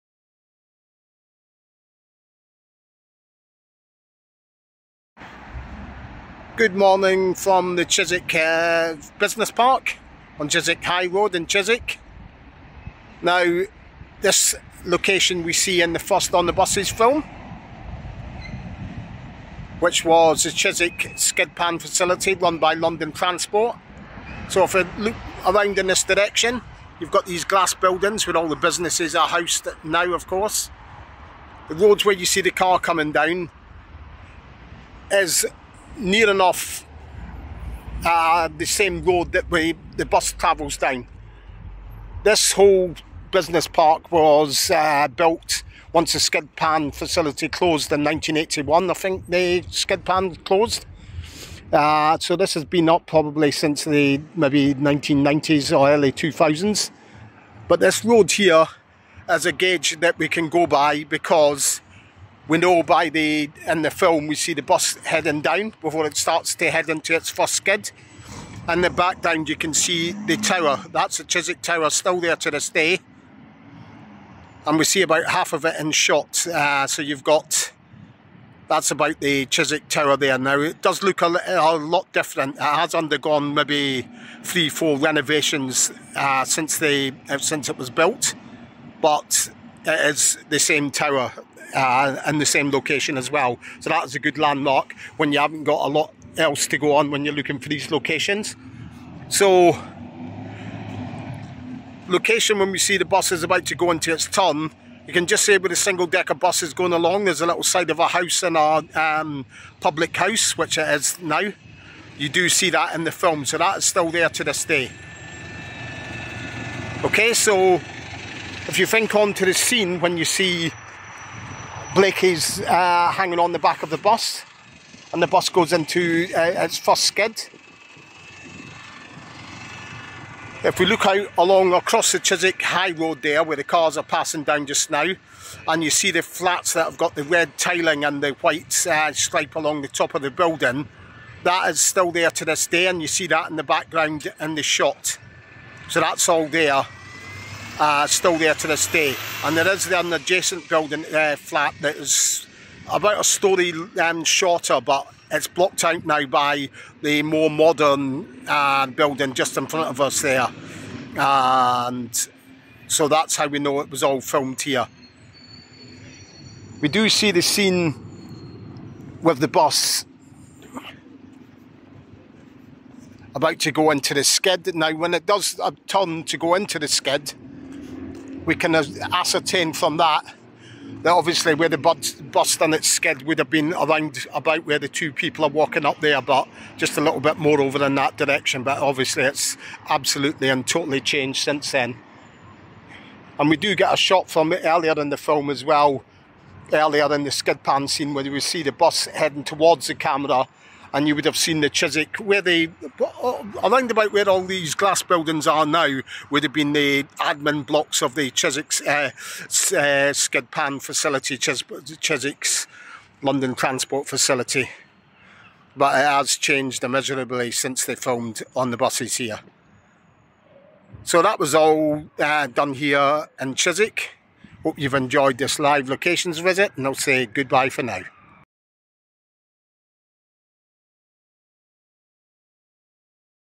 On the Buses (1971): Skid Pan Scene - A Live Locations Visit
Pop on a nostalgic trip to the former site of London Transport's Chiswick Skid Pan facility where an iconic scene from the first On the Buses spin-off film was made. See where the bus skids and Blakey falls from the back of the bus. Although the location has changed beyond recognition the clues are there.